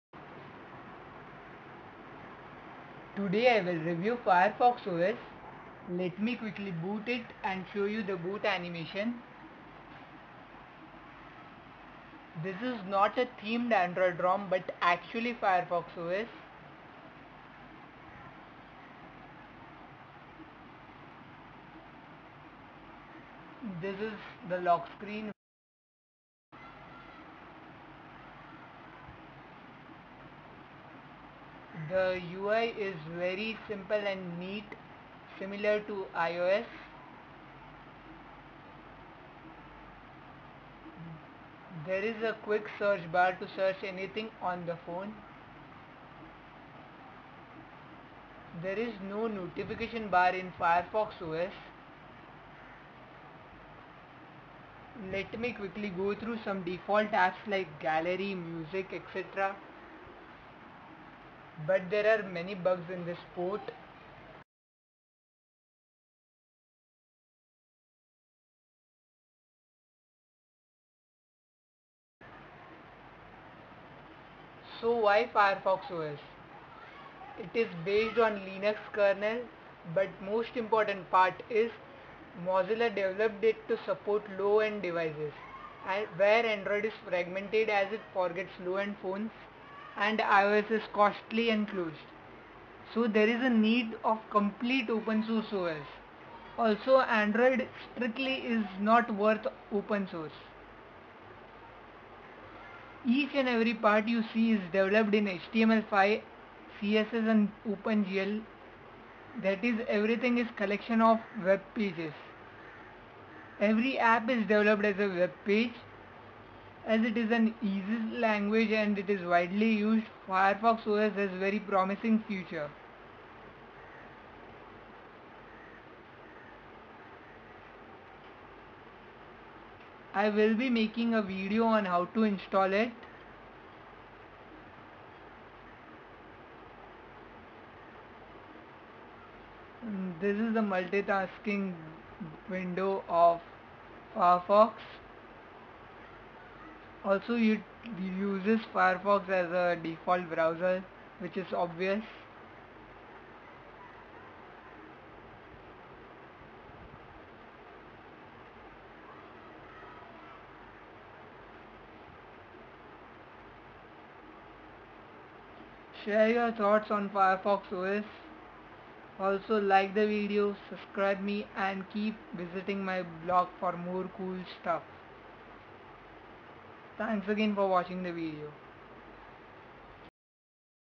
Mozilla FireFox OS Hands ON (on Android Device)
The HTC Explorer, or Pico receives Mozilla Firefox OS .The port comes from  XDA Recognized Developer cute_prince.

The "not working" list is as follows:

SIM card detection,
USB mass storage,
Video recording,
Bluetooth